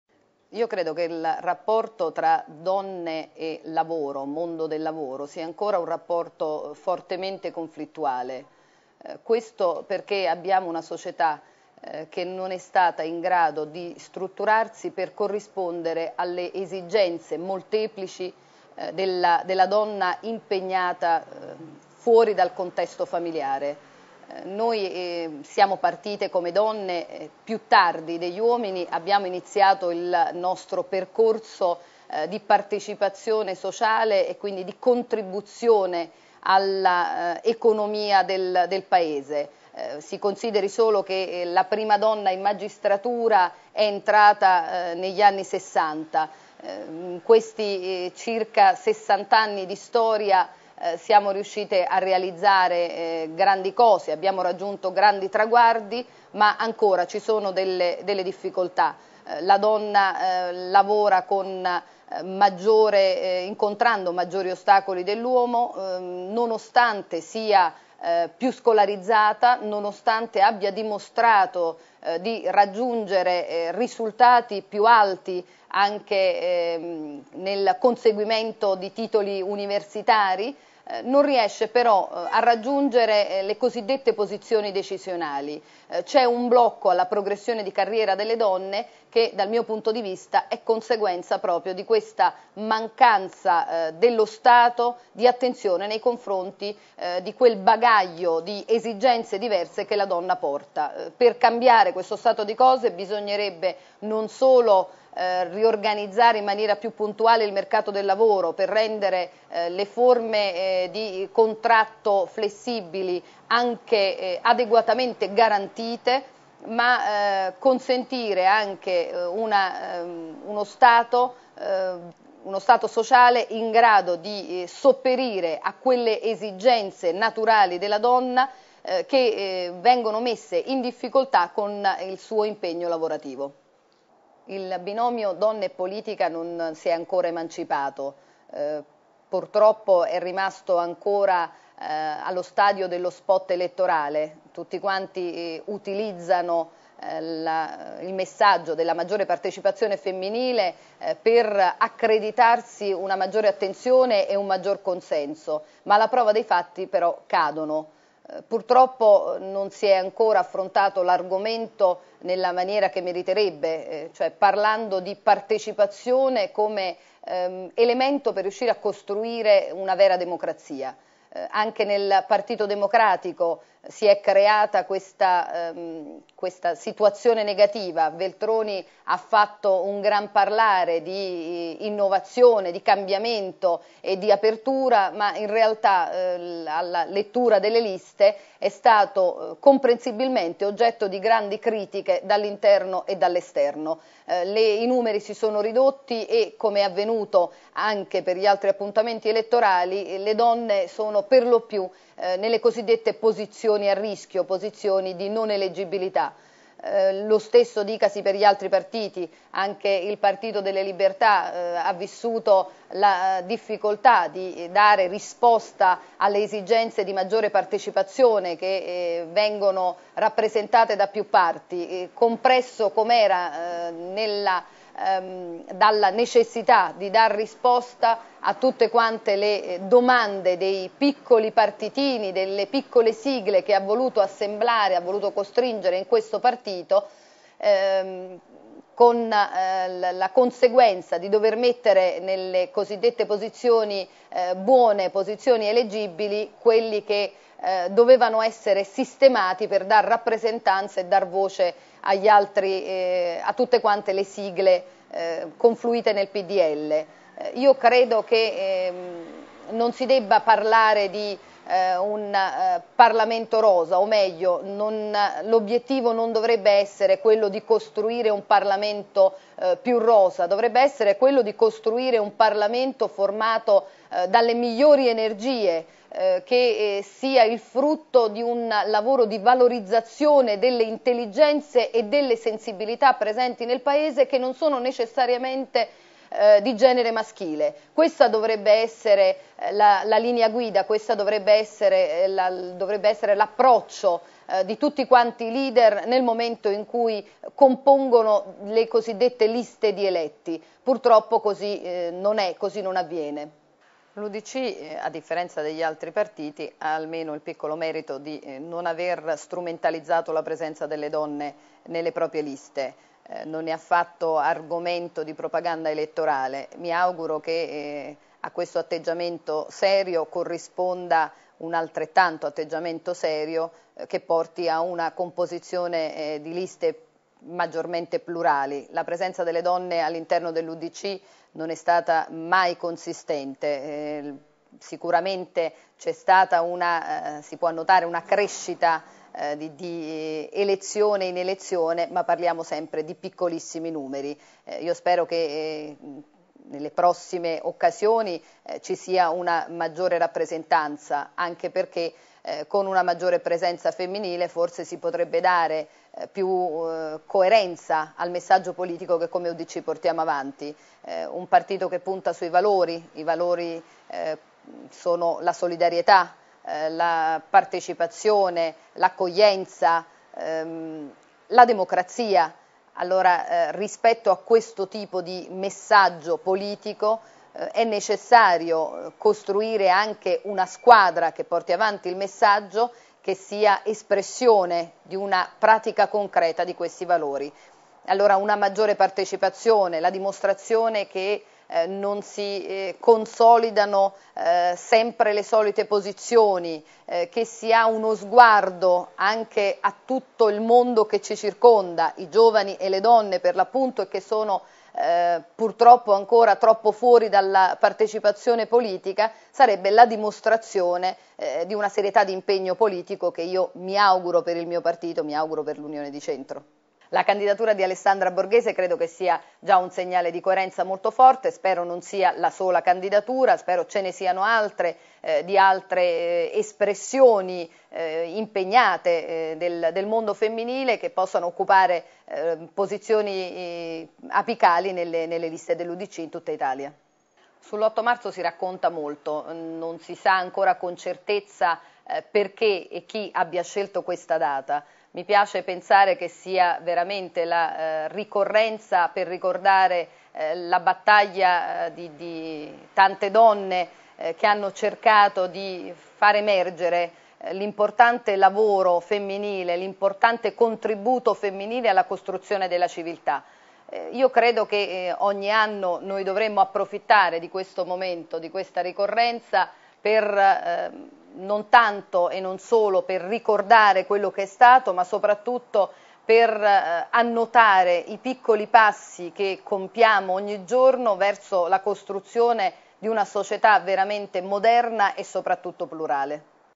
Ostacoli alle donne in carriera ma no quote, sì al merito
Erminia Mazzoni, Udc, esamina i problemi del lavoro femminile: «Segnali positivi dalla politica, purché non sia facile propaganda»